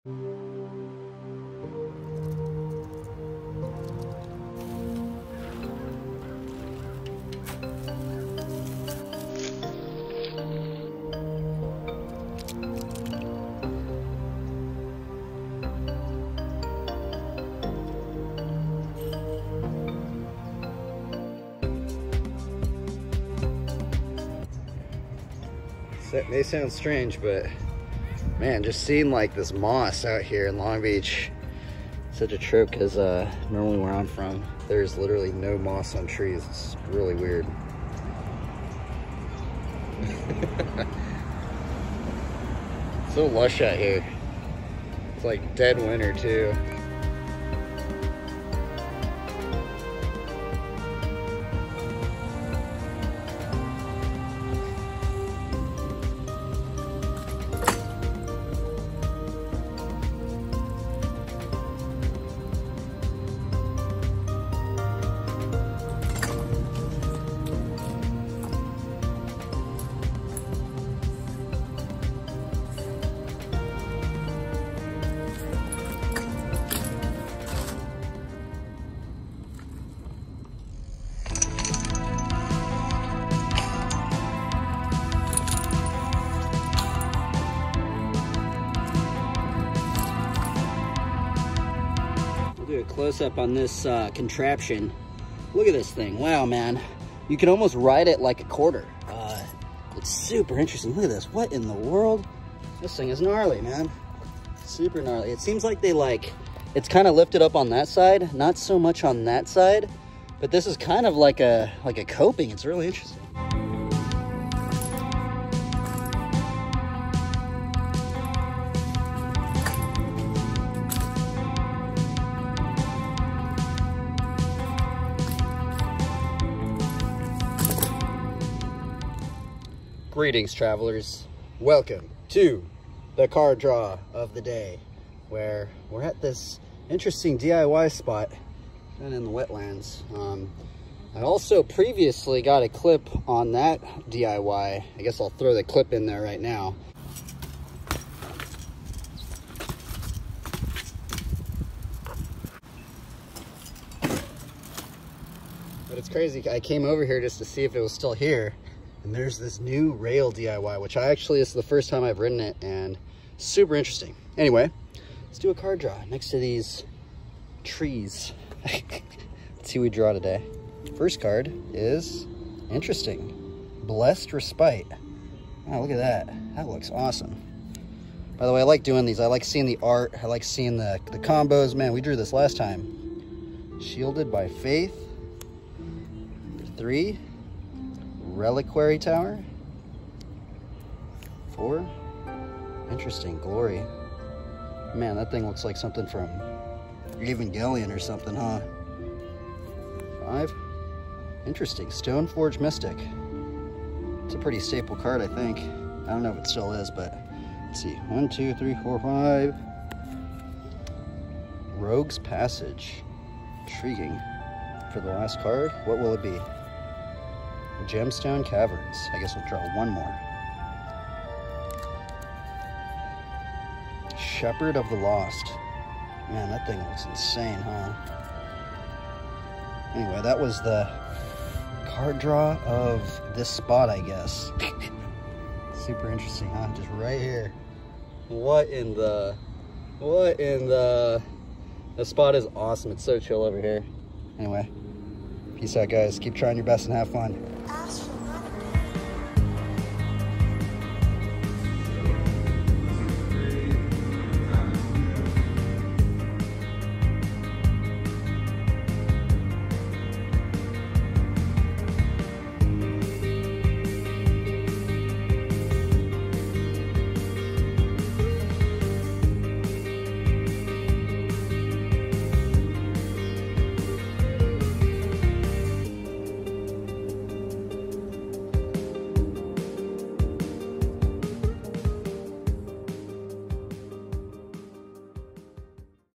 Long Beach D.I.Y and Tree Ride BMX Vlog
fun times in Long Beach probabaly the most ive ever filmed on a trip haha